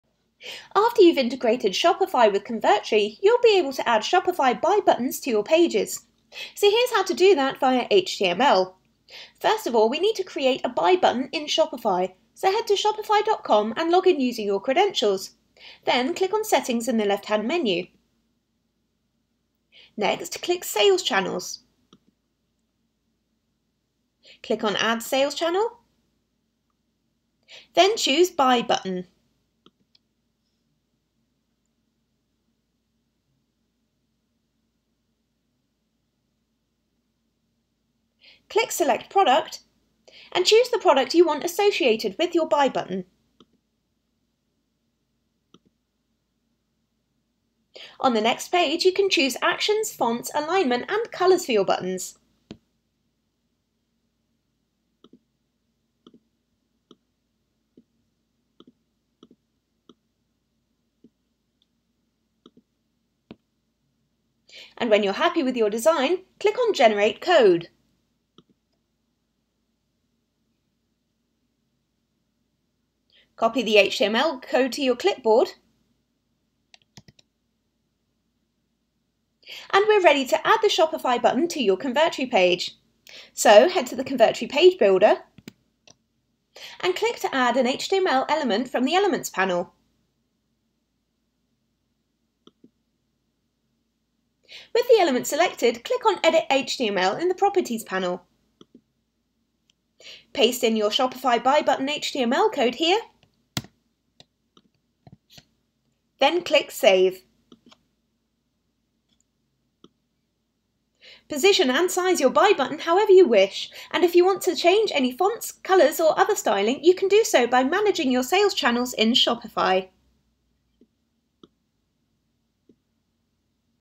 Convertri Help - How to Add a Shopify Button via HTML
There's two ways of adding a Shopify button: here's how to do it via HTML.